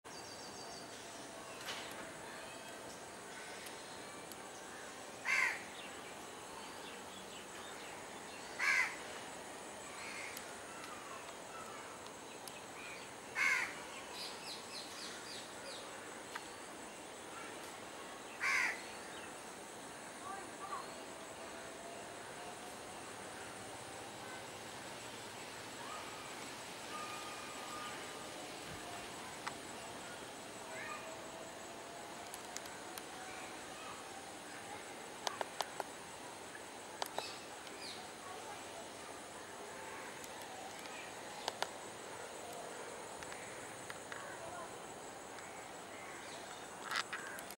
Blue Tiger Butterfly Video
Shrikant Madhav Kelkar's video clip taken at Kalwa (Thane), Maharashtra, India in December 2015

Scientific Name : Tirumala limniace